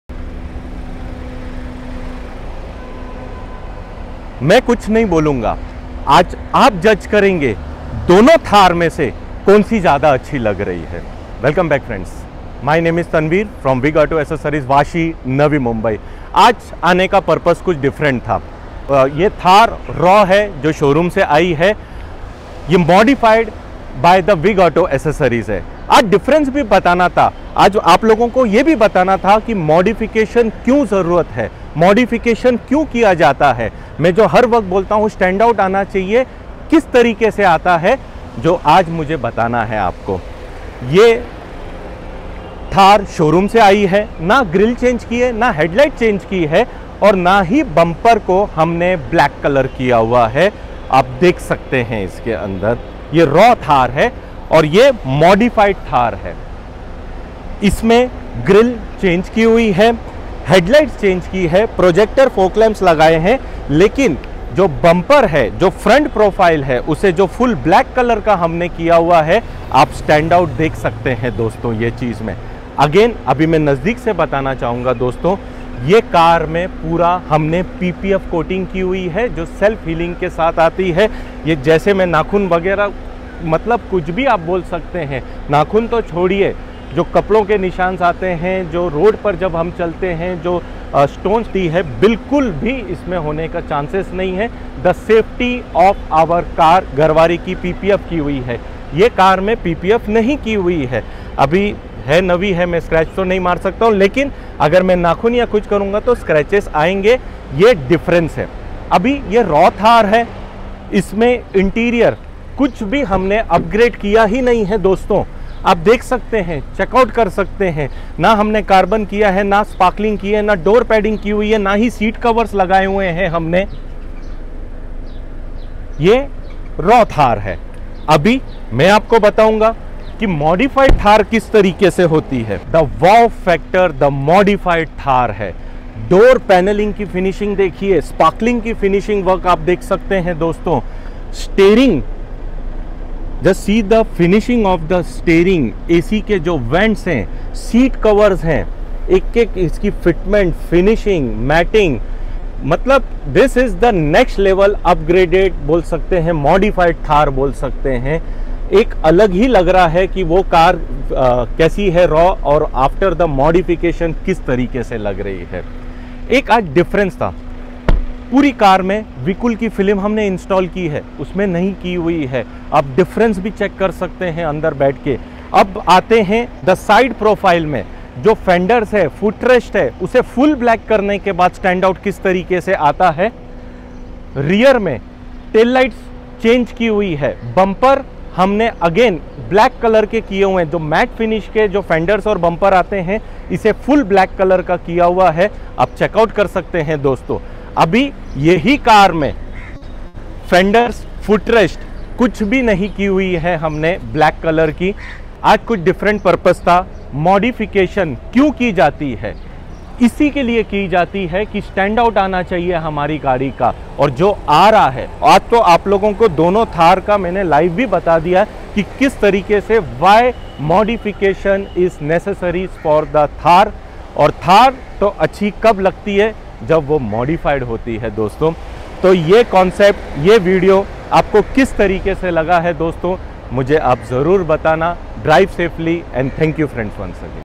Mahindra THAR Modified || Modified vs Stock || PPF || Why is Modification Important
Hi youtube family hope you are doing good!

Vig Auto accessories
Shop No. 3, Vardhman mkt, Sector 17, Vashi, Navi Mumbai, Maharashtra 400703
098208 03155